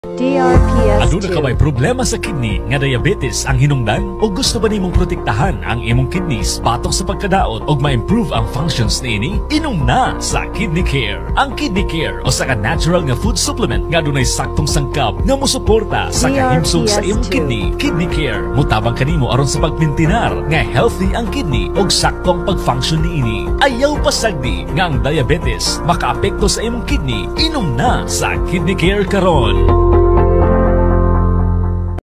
Kidney Care Radio Ad 2022-2024 32s
Ito po ang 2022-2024 Radio Ad ng Kidney Care Food Supplement.

DRPS Two 6154: Kidney Care Radio Ad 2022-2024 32s